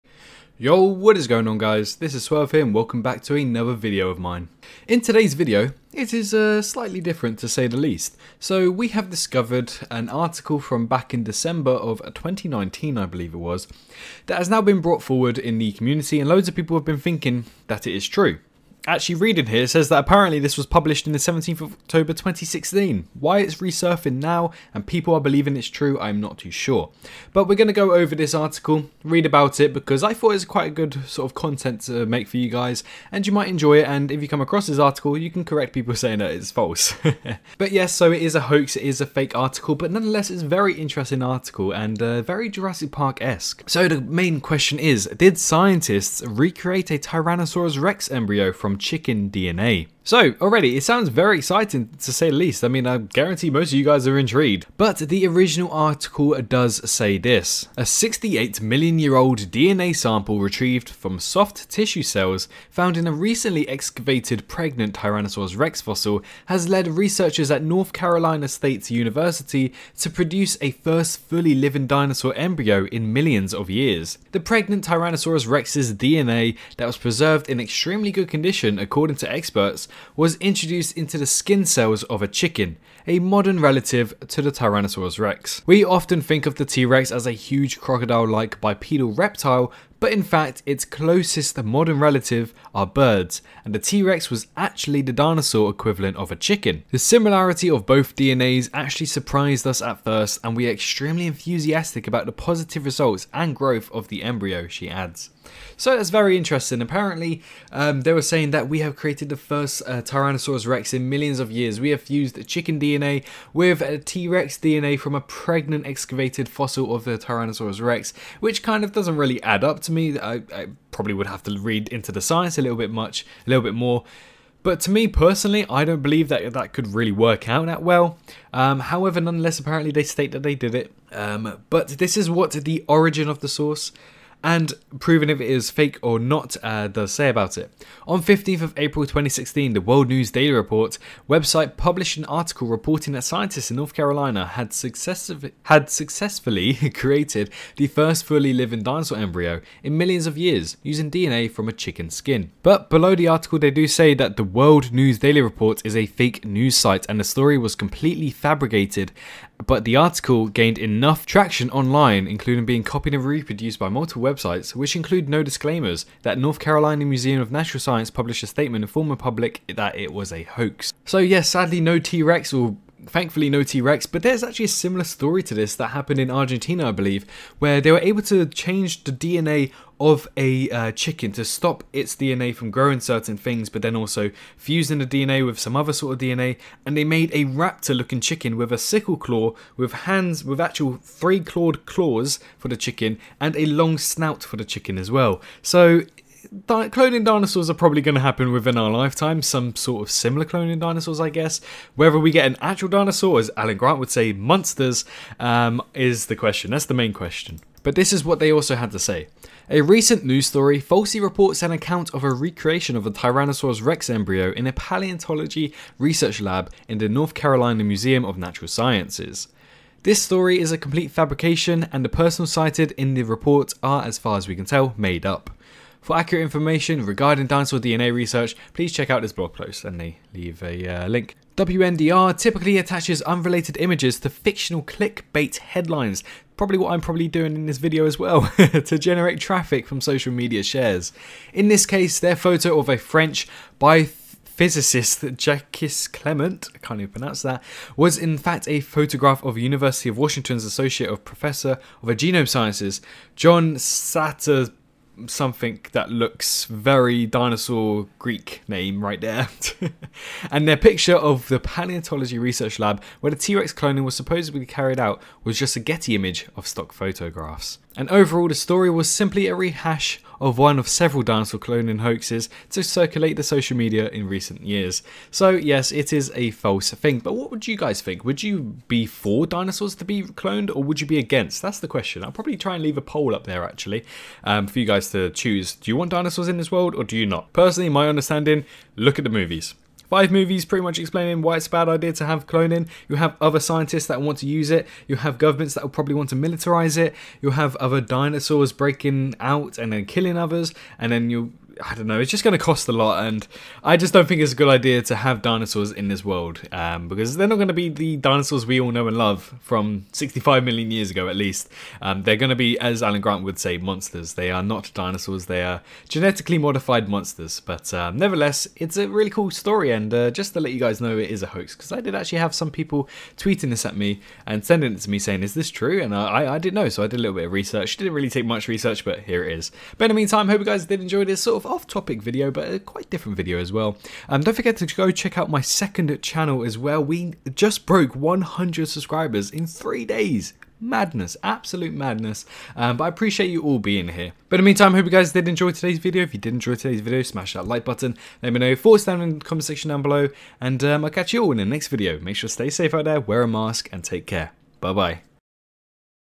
Did Scientists Recreate a T-Rex from a Chicken Embryo? - Dinosaur Discoveries
In this video we'll be debating if scientists actually recreated a T-rex in an embryo of a chicken from an unusual 2016 article!